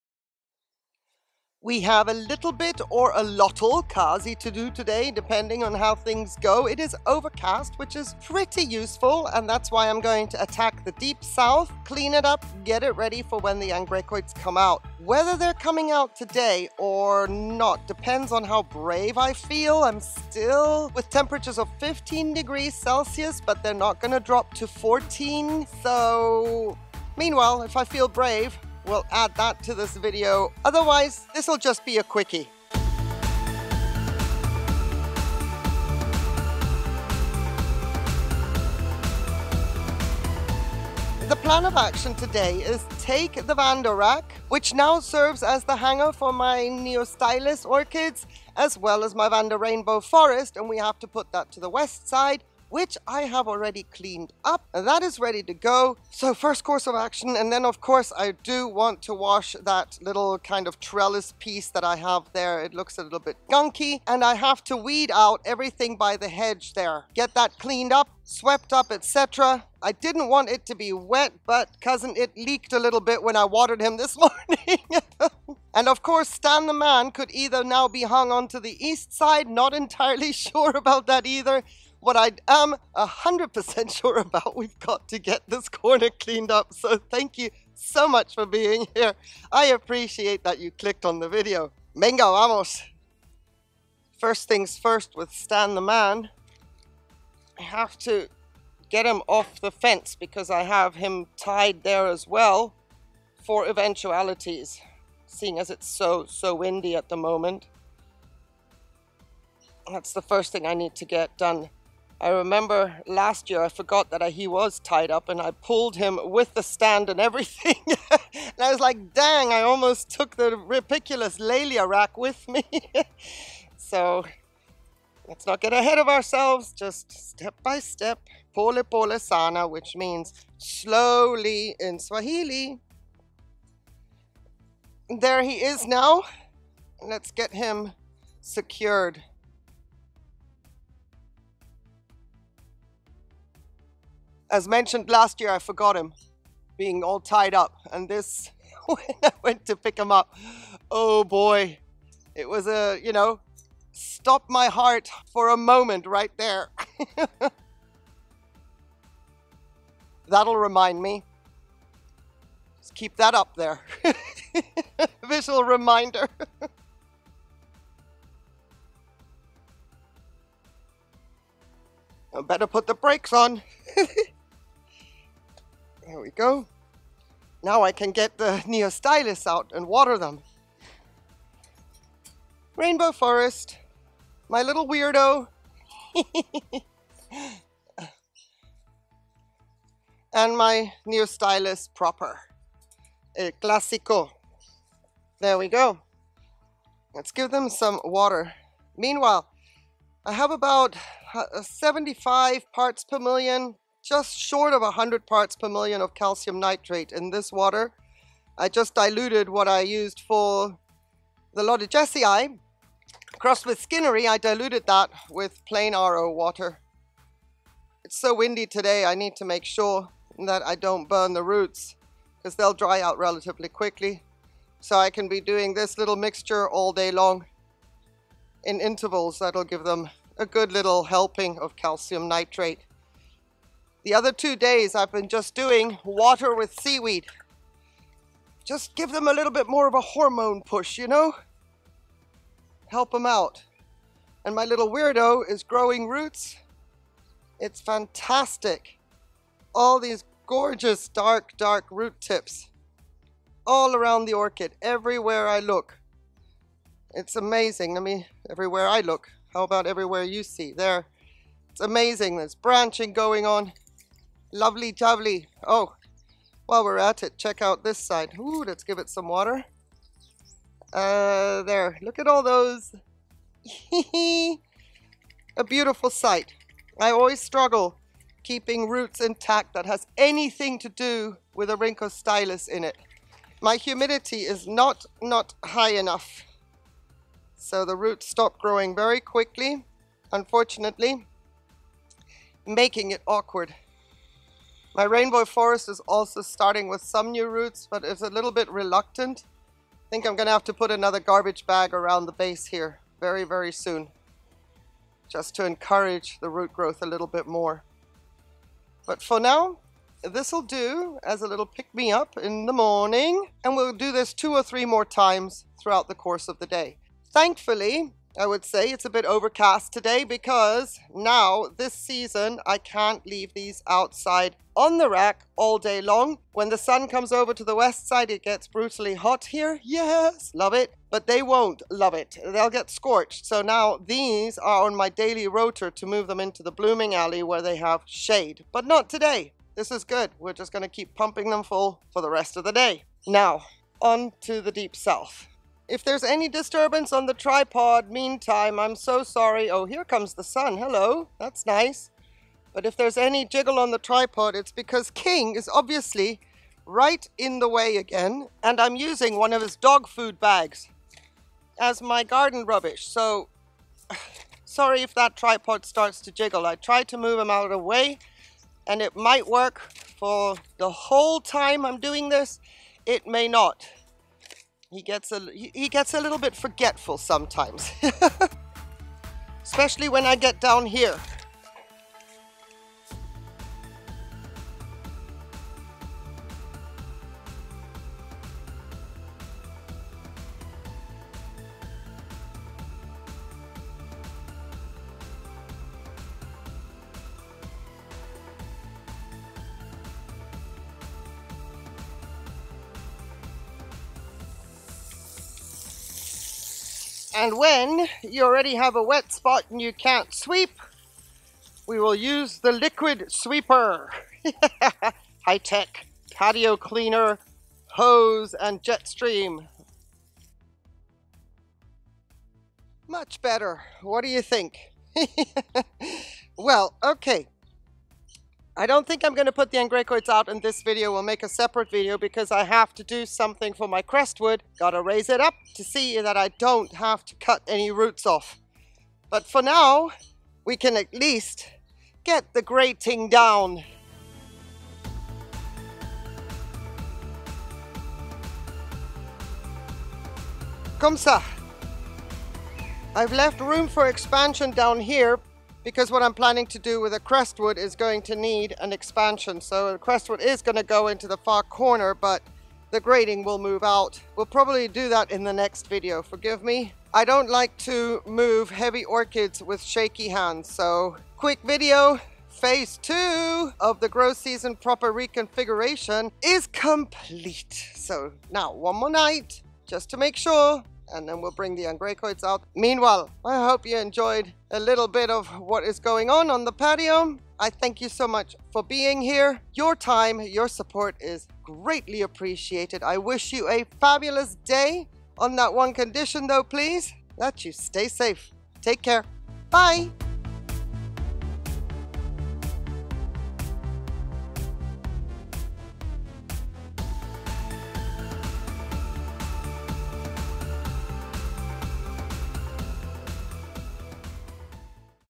Cleaning up The Patio Deep South | Preparing Season 2023 Angraecoid Grow Space
Soooo close! We are so close to getting the angraecoids out for their summer glam camping sesh of 2023. The night temperatures are steadying out at 15ºC / 59ºF, and while I believe I could have brought the Angreacum Crestwood 'Tomorrow Star' and Angreacum sesquipedale var. bosseri out, I am reluctant to do it too soon! They have been indoors since Mid November 2022, and it won't make a difference to have them protected for another night. Spring has a way of being bipolar 😆
My environment:
Mediterranean climate, hot summers / mild winters
45% - 85% humidity, with regular airflow
Thumbnail / Outro Image:   Bodum.westernscandinavia.org
Orchid growing supplies:
               Seramis, Akadama
Water: Reverse Osmosis
Insecticidal Soap
General PPM: 300 Summer, or in active growth
                          150 Winter, RO Water and KelpMax only